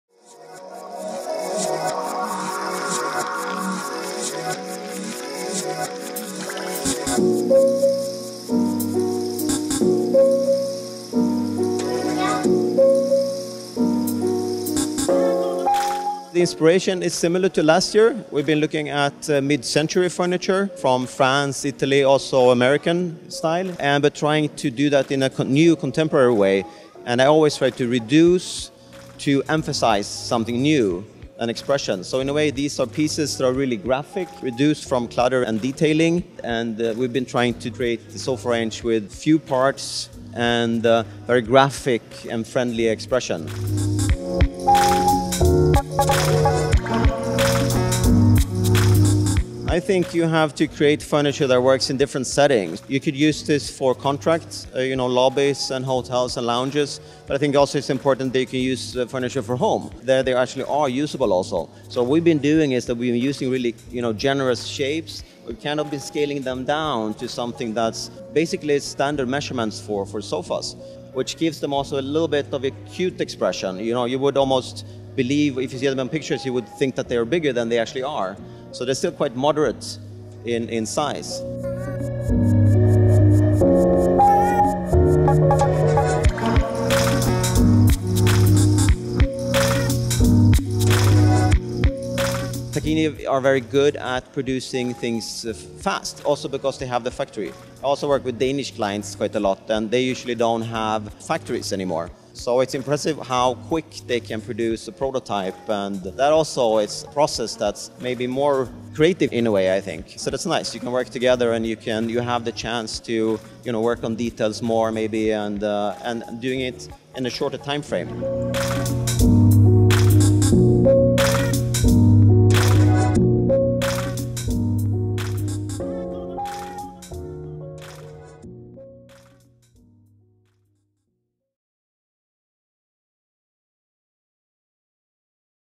Julep Sofa
Jonas Wagell presents the Julep Sofa.

Source: Archiproducts